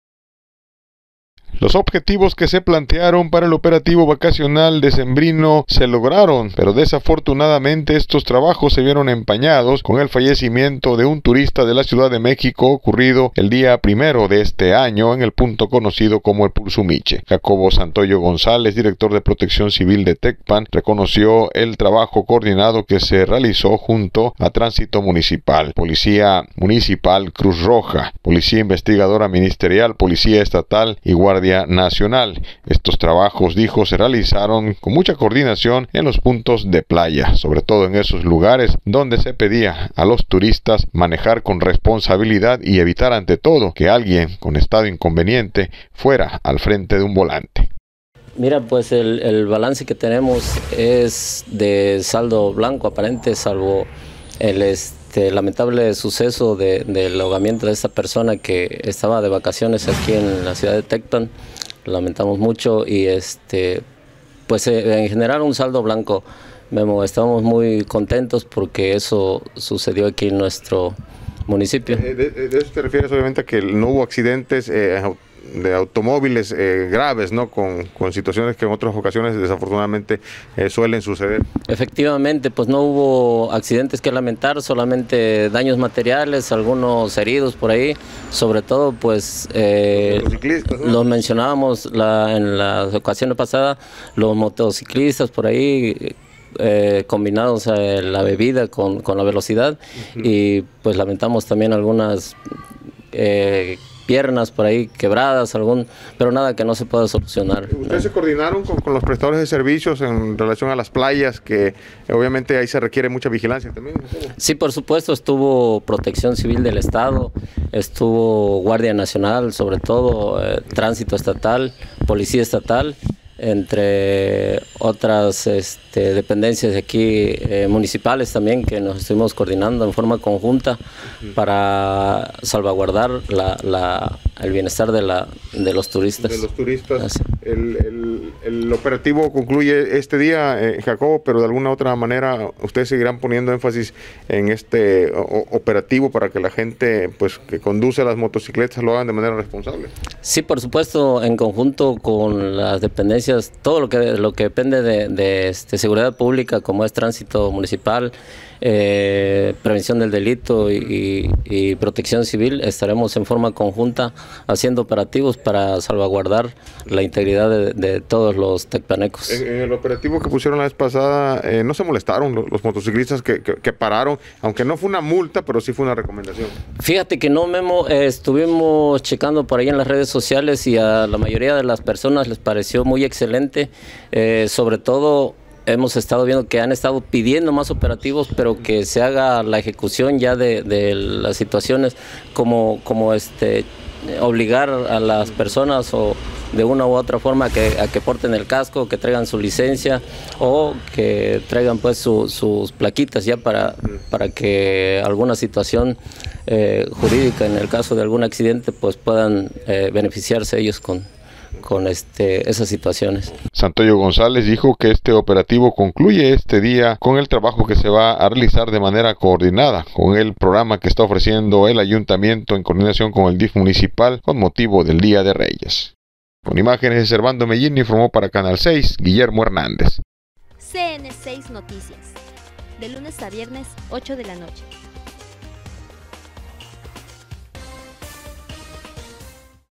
Se lograron los objetivos del operativo vacacional decembrino.
Se lograron los objetivos del operativo vacacional decembrino informó el director de PC de Tecpan Jacobo Santoyo